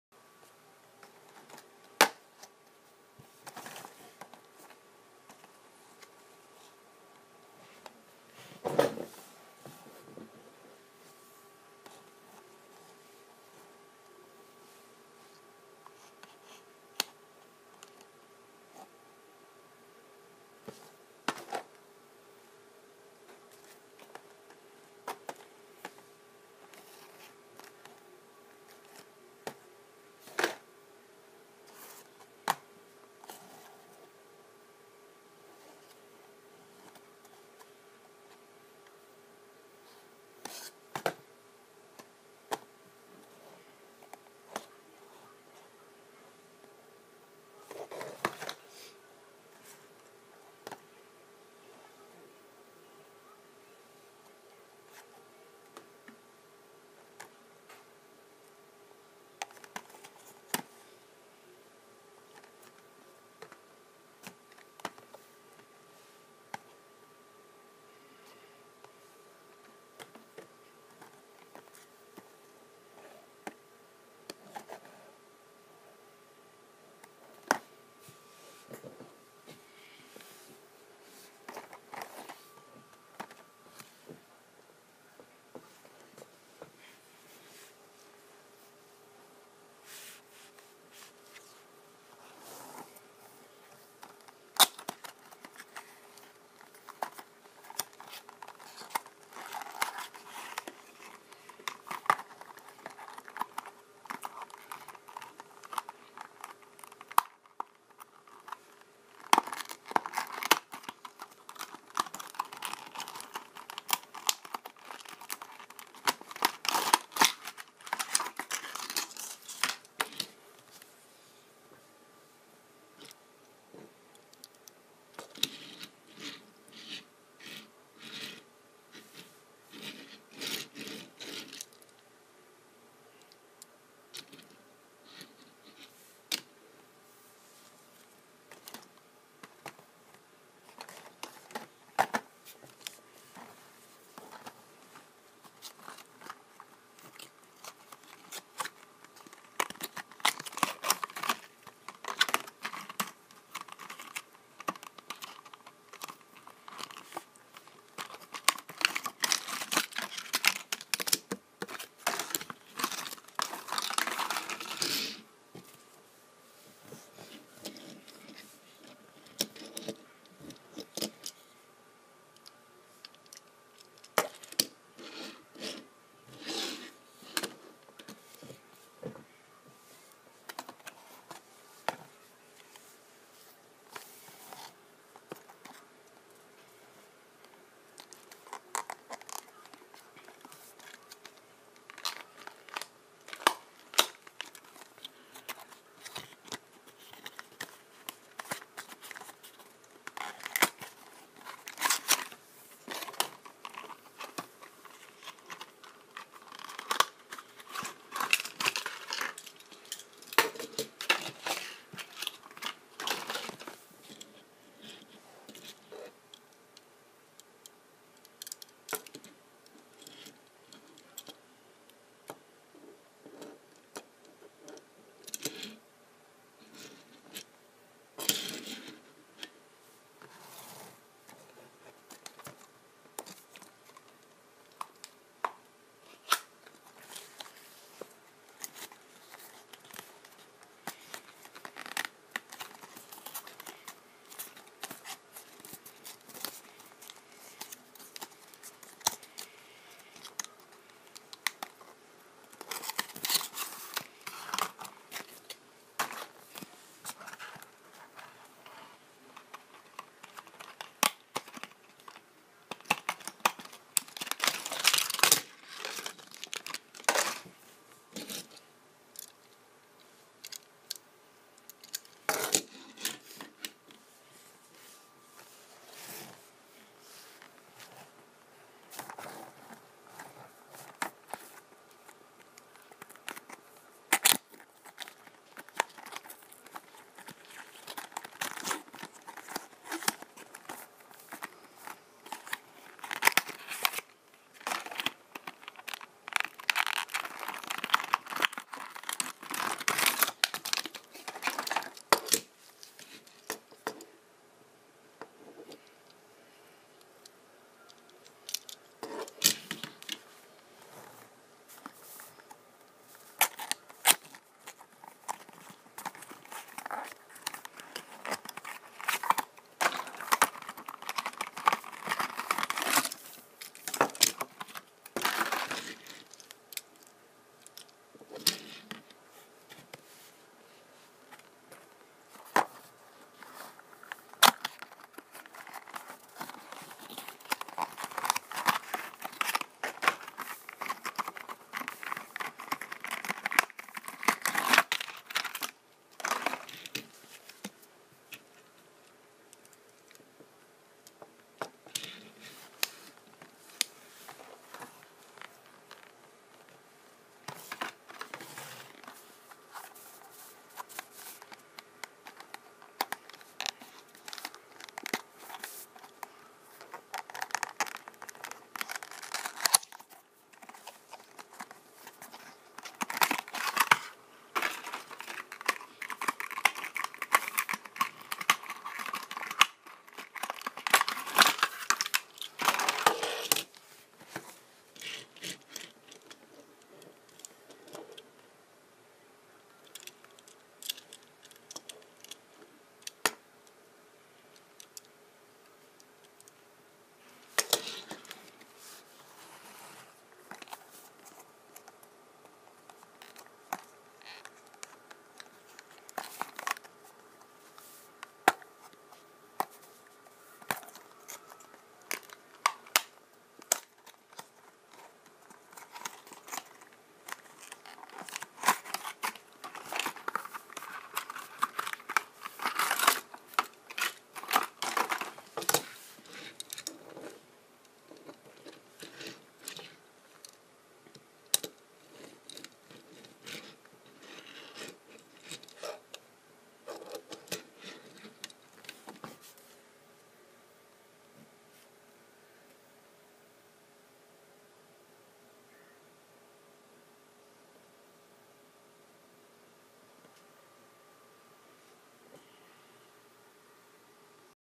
Stuff I Got From The 2014 Federated Auto Parts 400
Do you like it? I met Kyle Larson at Richmond. And I got all these cars and unboxed them.